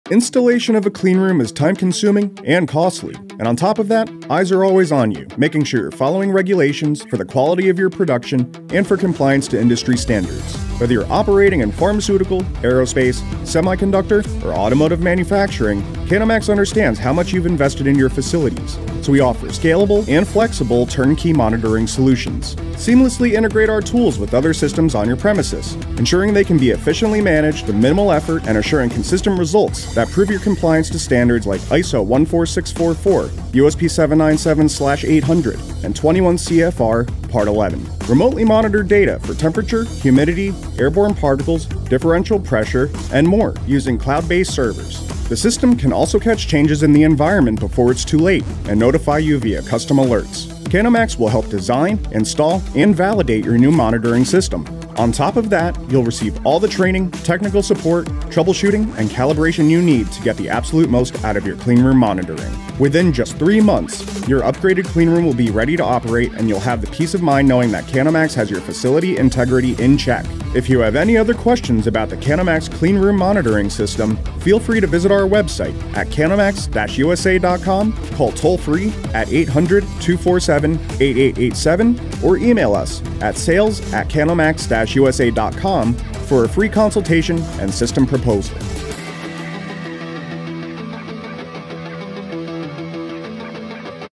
Introducing the Kanomax Cleanroom Monitoring System | Advanced Environmental Control
Discover the innovative Kanomax Cleanroom Monitoring System, designed to deliver precise, real-time environmental data for your cleanroom or controlled environment. This cutting-edge system ensures optimal performance and compliance with industry standards by continuously monitoring crucial parameters like particle count, airflow, temperature, humidity, and differential pressure.

Whether you’re in pharmaceutical manufacturing, healthcare, or aerospace, Kanomax provides unparalleled reliability and accuracy to protect your critical processes. Watch this video to see how our monitoring solution simplifies compliance, enhances productivity, and safeguards your sensitive environments.

🔹 Key Features:
- Real-time monitoring and alerts
- User-friendly interface and seamless integration
- Industry-standard compliance and certifications
- Scalable design to meet your facility's needs

💡 Learn how Kanomax can help you maintain a pristine cleanroom environment with confidence and ease.